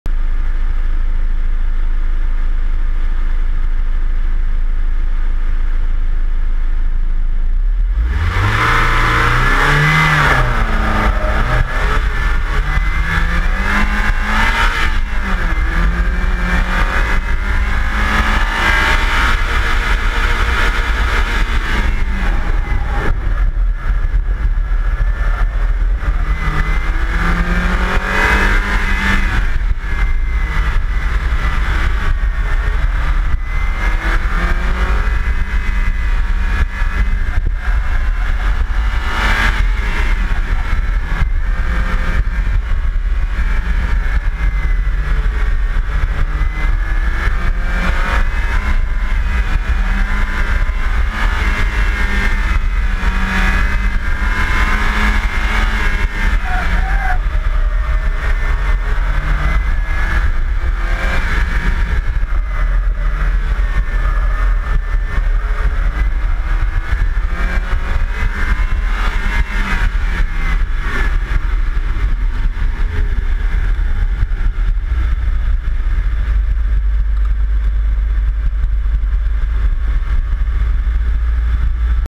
2016 ORSCCA Solo Event 9 - Rev Limiter Action
How much rev limiter is too much? This was my fast run of the day in Packwood, WA at the 2016 SCCA Oregon region autocross event #9.